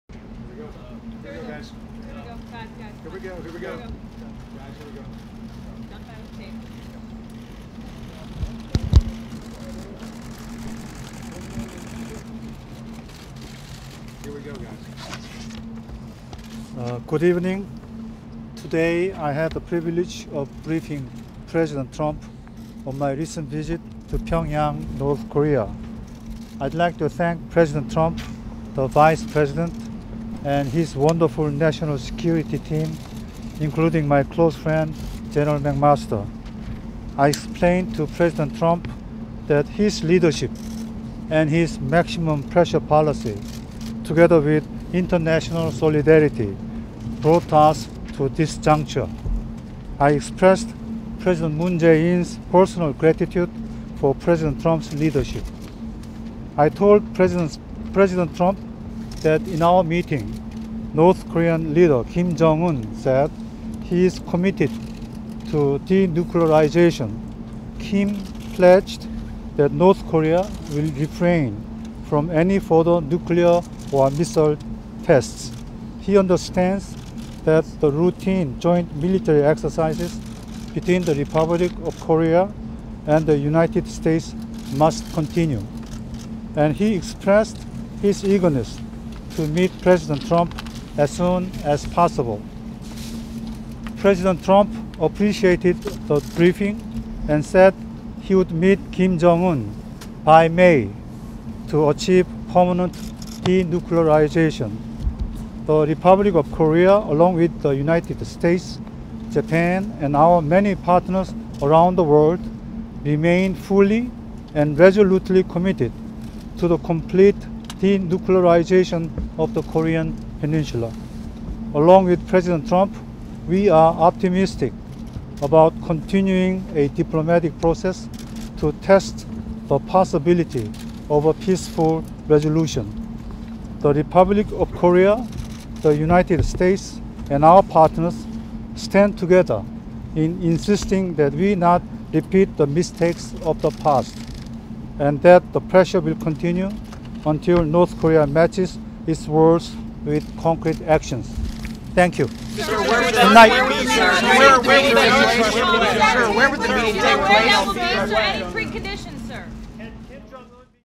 Trump To Meet With North Korea Leader
March 08, 2018 - South Korea's National Security Adviser Chung Eui-yong said President Donald Trump and North Korean Leader Kim Jong Un will meet by May this year. (March 8)